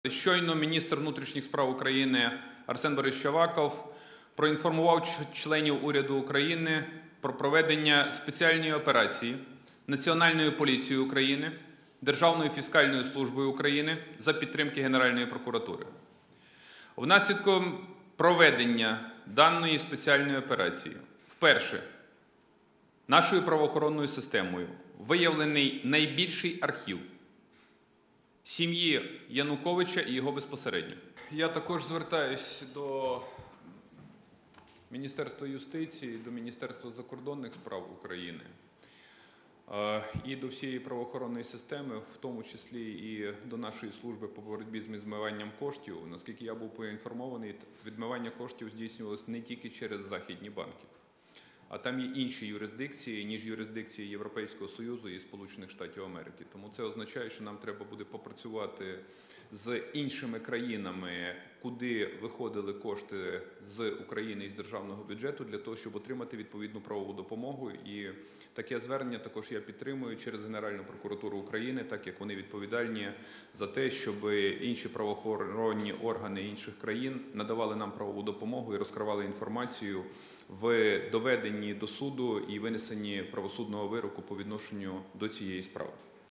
Нацполіція та ДФС за підтримки ГПУ виявили найбільший архів Януковича та його «сім'ї»
Внаслідок спецоперації Національної поліції та ДФС за підтримки Генеральної прокуратури виявлено найбільший архів Януковича та його «сім'ї».

Про це повідомив Прем’єр-міністр України, лідер Політичної партії «Народний фронт»  Арсеній Яценюк на засіданні Кабінету Міністрів у середу, 16 грудня.

«Щойно Міністр внутрішніх справ України Арсен Аваков проінформував членів Уряду України про проведення спеціальної операції Національною поліцією України, Державною фіскальною службою України за підтримки Генеральної прокуратури. Наслідком проведення даної спеціальної операції вперше нашою правоохоронною системою виявлений найбільший архів «сім'ї» Януковича і його безпосередньо», - наголосив Арсеній Яценюк

Заступник Голови Національної поліції України, в.о. Голови Нацполіції Олександр Вакуленко, доповідаючи на засіданні Уряду нагадав, що слідчими Національної поліції за матеріалами Державної фіскальної служби було розпочато кримінальне провадження за фактом діяльності низки конвертаційних центрів, які займалися в тому числі і відмиванням бюджетних коштів. У ході реалізації отриманої оперативної інформації 15 грудня слідчими спільно з оперативними працівниками на одній з приватних квартир в місті Києві було проведено обшук, у ході якого вилучено значну частину «архіву сім'ї».

Арсеній Яценюк підкреслив, що за інформацією правоохоронних органів, відмивання коштів здійснювалося не тільки через західні банки: «Там є інші юрисдикції, ніж Європейського Союзу і Сполучених Штатів Америки».

Глава Уряду звернувся  до Міністерства юстиції України, Міністерства закордонних справ України і до всієї правоохоронної системи, в тому числі Служби по боротьбі з відмиванням коштів, наголосивши на необхідності отримання правової допомоги інших країн, щоб з’ясувати, «куди виходили кошти з державного бюджету України».

Арсеній Яценюк підкреслив, що підтримує ініціативу щодо направлення відповідного  звернення через Генеральну прокуратуру України, «адже ГПУ  відповідальна за те, щоби правоохоронні органи інших країн розкривали інформацію і надавали нам правову допомогу в доведенні до суду і винесенні правосудного вироку по відношенню до цієї справи».

Приєднуйся!